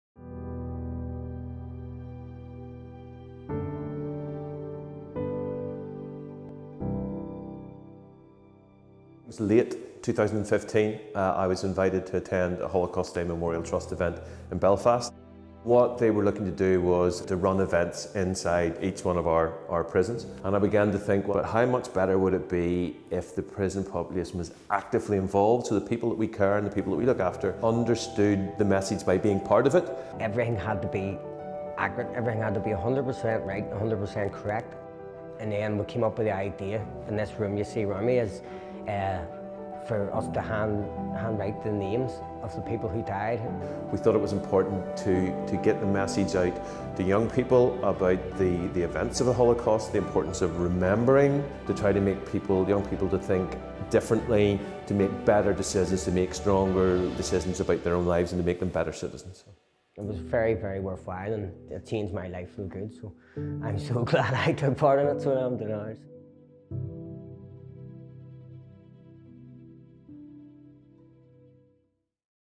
The impact of Holocaust Memorial Day in Northern Ireland
At HMP Magilligan, learners in the prison’s education programme worked together to create an onsite museum and memorial to the Jews killed in the Holocaust. Find out how the project was developed and what taking part meant to the prison population.

Find a local HMD activity that you can attend near you on our interactive activity map